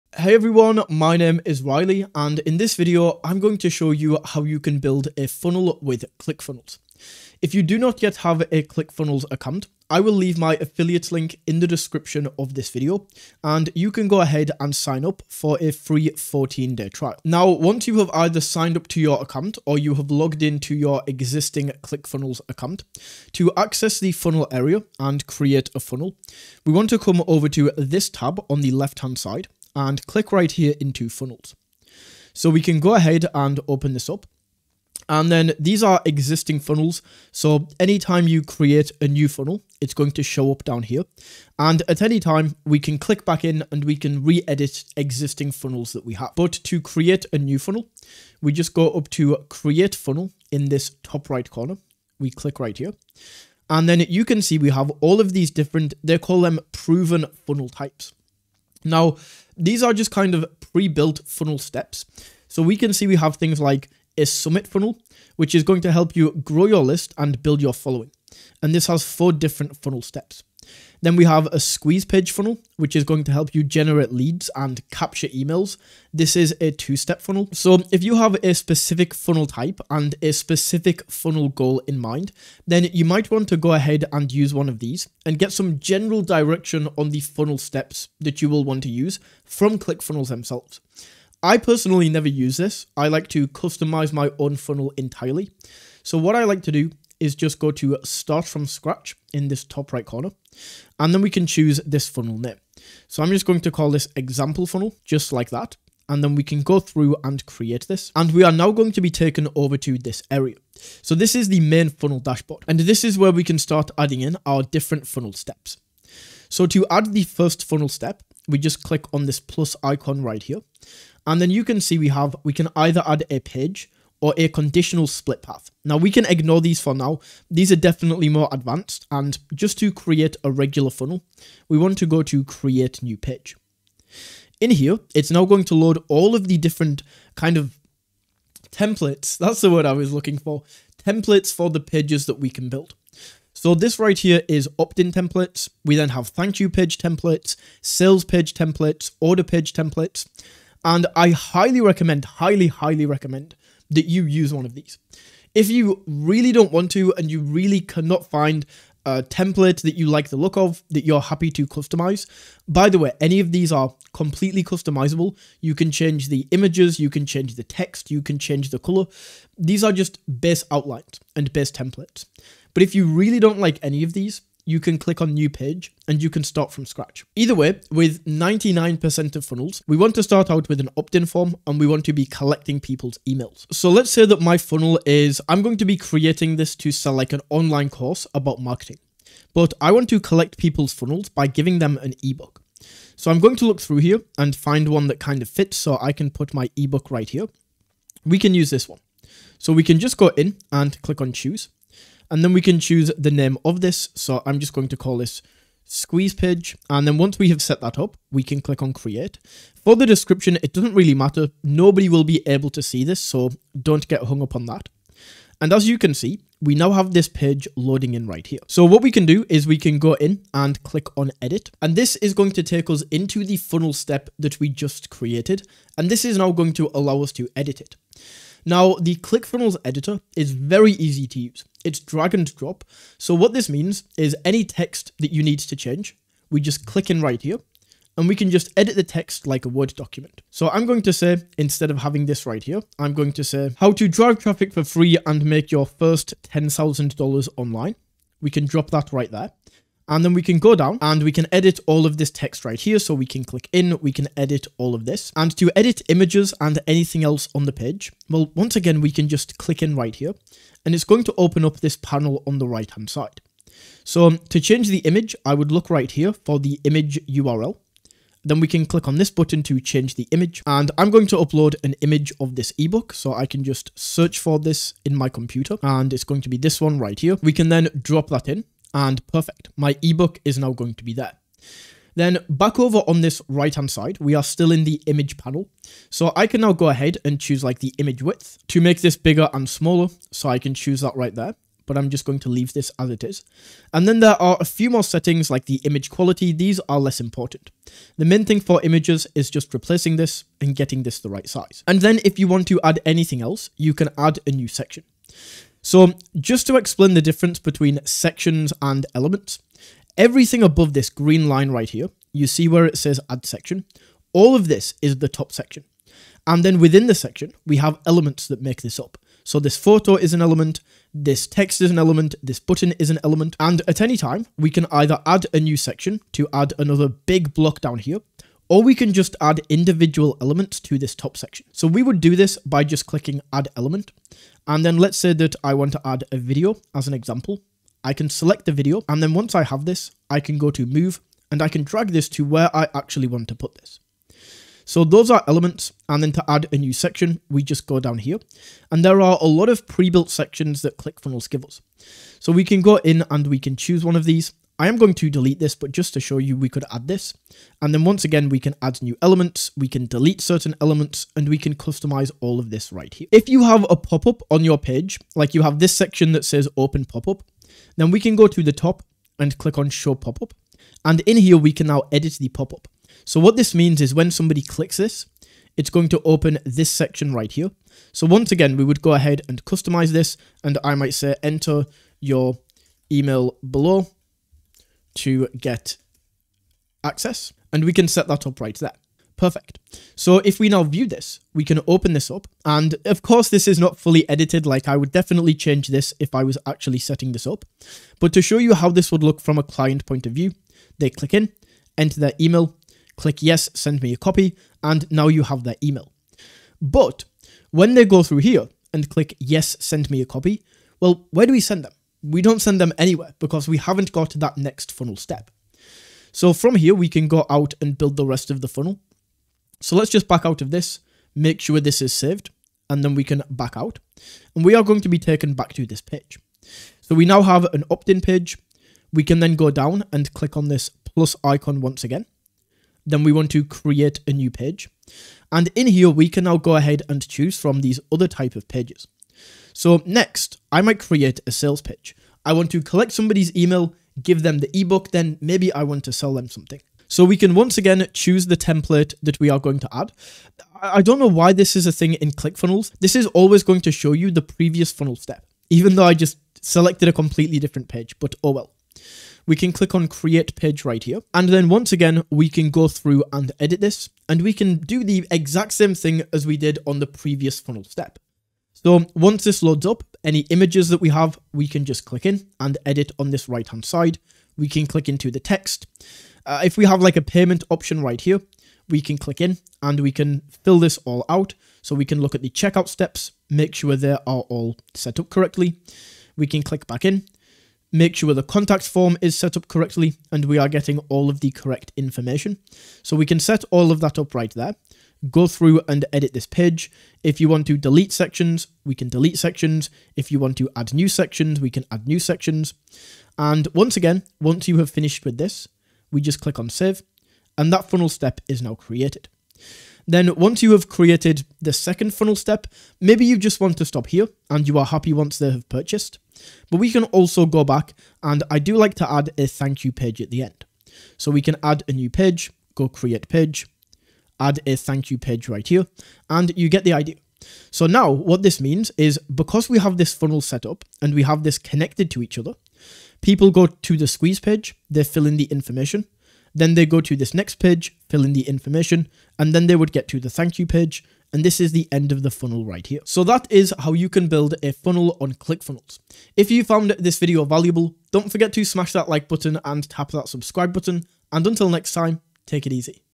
How to Build a Funnel in Clickfunnels 2.0 (2024) | Step-by-Step Tutorial For Beginners
In this video, I show you step by step how you can create a funnel on Clickfunnels, from creating the funnel steps, adding a squeeze page, sales page and thank you page to customizing all of your pages. This video will take you from beginner to expert at building funnels.

🚀 Ready to build your Ecom or Expert Business? Take the all-new OFA Challenge!